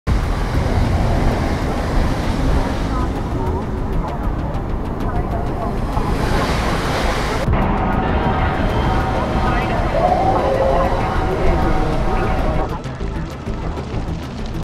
Cyberpunk City Environments - Blender
1 year old cyberpunk city environments made with Blender for an ad that never got of the ground.